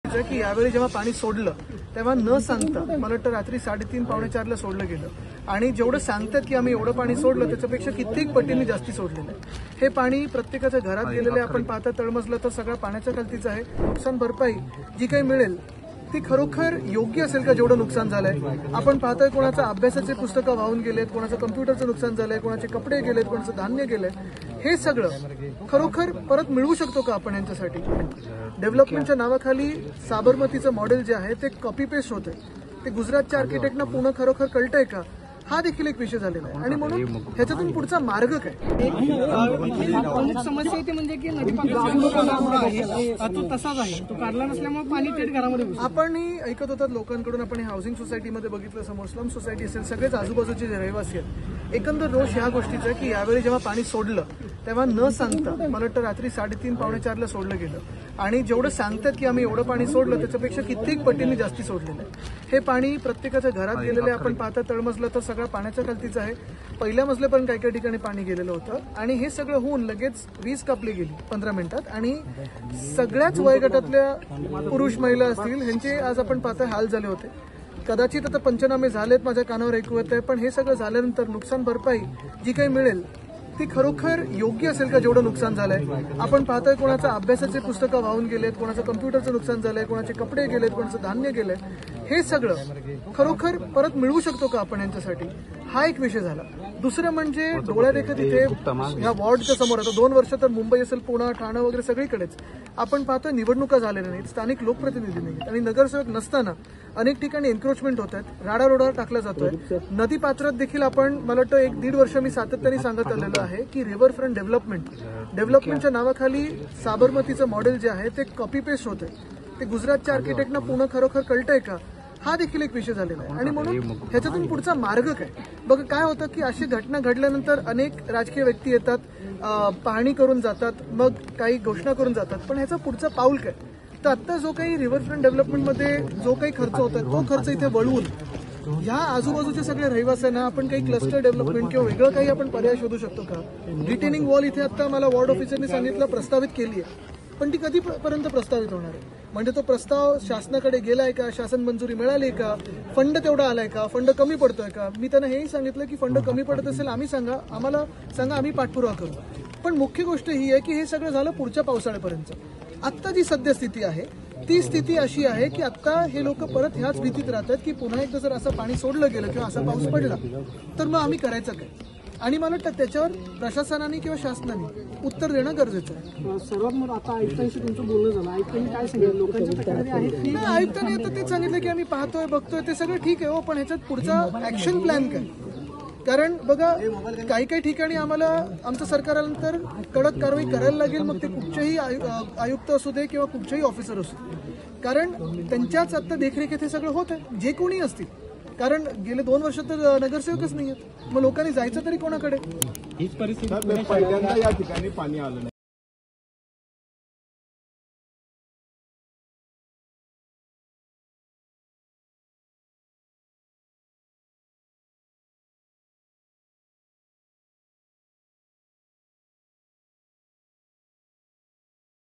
Aditya Thackeray : 'त्या' नागरिकांचं झालेलं नुकसान भरून निघेल का? ठाकरेंचा सरकारला सवाल... | LetsUpp
पुण्यातील सिंहगड रस्ता भागातील नागरिकांना सांगितलं त्यापेक्षा जास्त पाणी धरणातून सोडण्यात आले. त्यानंतर लगेचच वीज पुरवठा खंडित करण्यात आला. तळमजला, पहिला मजला पाण्यात गेल्यामुळे नागरिकांचे मोठ्या प्रमाणात नुकसान झाले. पंचनामे झाले; मात्र यातून नागरिकांचे झालेले नुकसान भरून निघेल का? असा सवाल शिवसेना ठाकरे गटाचे आमदार आदित्य ठाकरे यांनी केला.